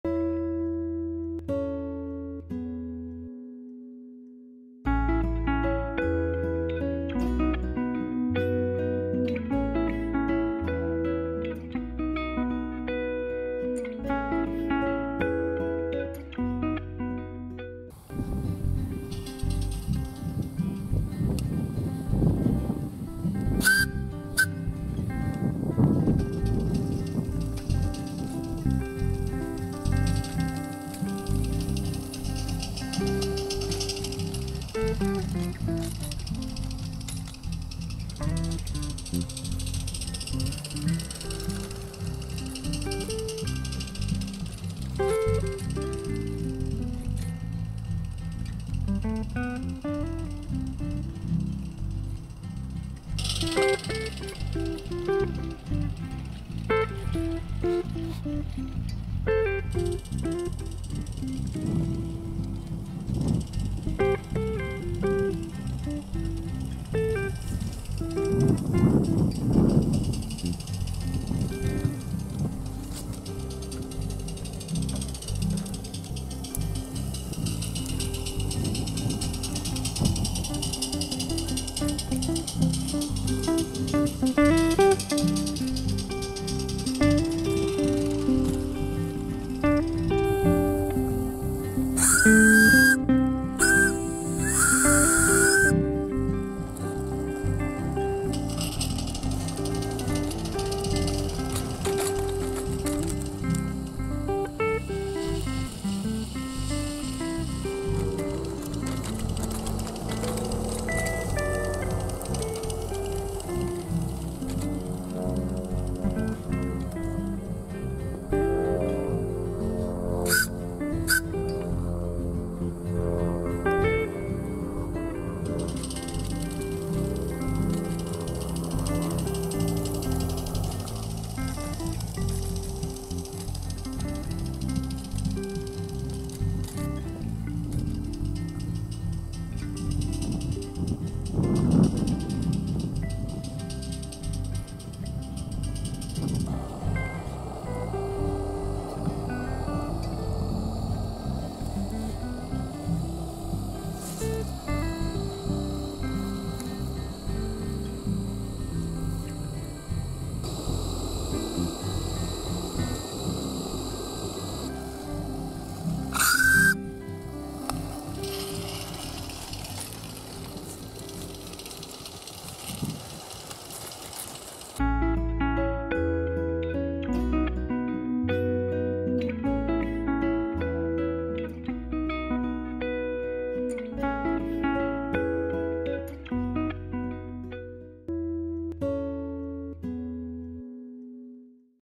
LGB Gartenbahn "Werkstattfahrten im Winter" - Regner Nr. 22
Werkstattfahrten im Winter bei der BKB - Echtdampflok Regner Nr.22, ein C-Kuppler, nach der Instandsetzung mit neuem Kessel von Reppingen DMR. - In winter, it's time to check and repair everything. Workshop trips are then available on the open track of the WKB. Today, the Regner 22 makes a first ride with a new one
Boiler by Reppingen (DMR).